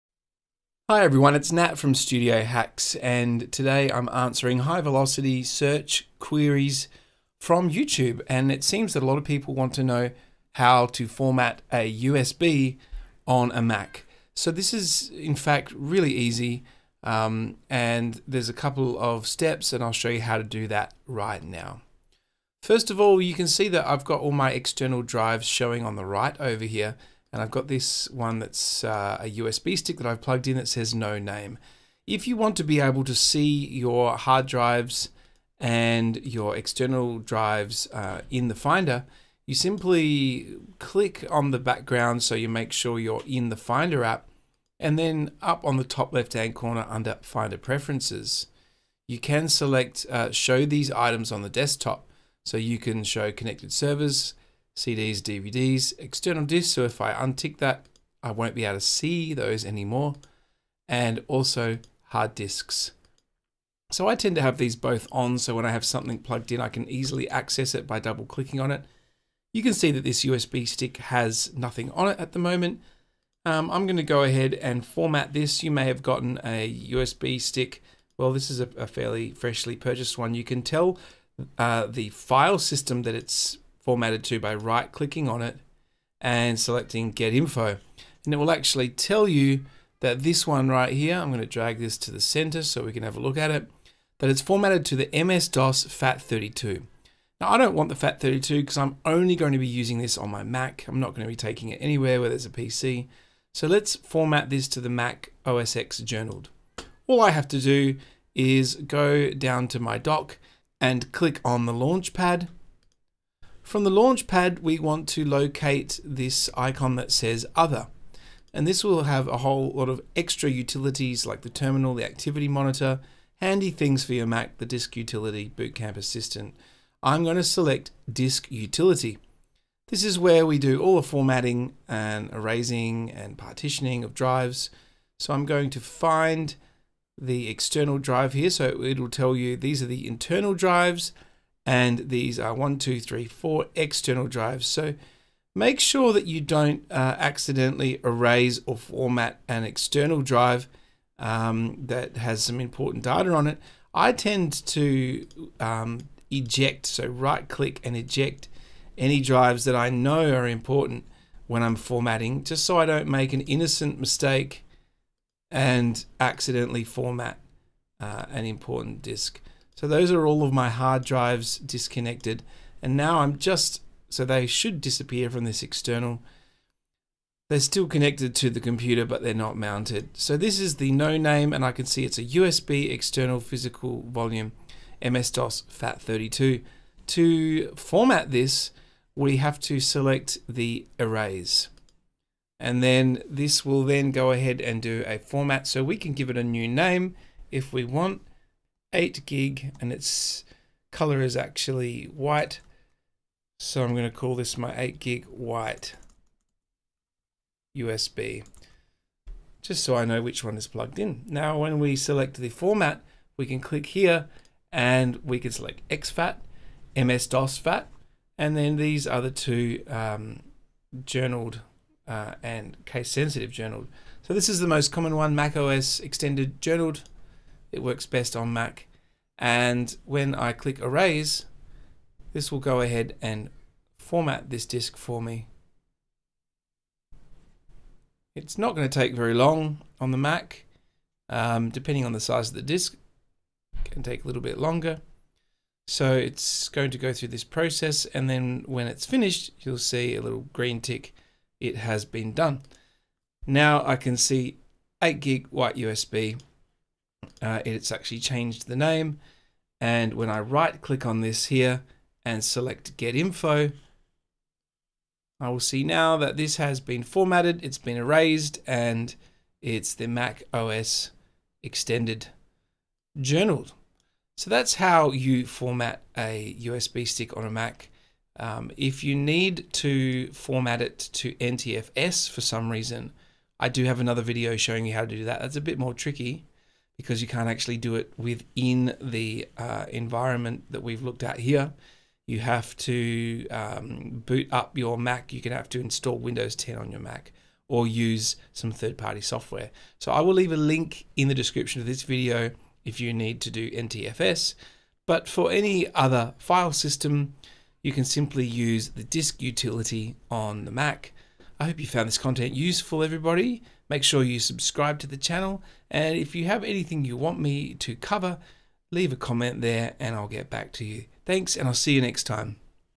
How To Format A USB On Mac (Flash Drive Format)
1. Connect the flash drive that you want to format.

2. Go to Applications and Utilities and launch Disk Utility. The application will appear on the screen, and you will see the main window, with a list of available storage devices on the left and various disk management options on the right. At the bottom of the main window are storage device details, including disk description, connection type, USB serial number, total capacity, write status, S.M.A.R.T. status, and partition map scheme.

3. Select your storage device from the list on the left and click on the Erase tab. Before you can click on the Erase button located in the Erase tab, you must select which file system you would like to use and give it a name. If you’re not sure which option you should choose, continue reading this article to find out more information about the main file systems used by macOS and Windows.

4. With everything set, you may click on the Erase button to start the formatting process. Depending on the size and speed of your flash drive, it may take a minute or two. Once the formatting process is finished, you should be able to transfer files to the newly formatted flash drive.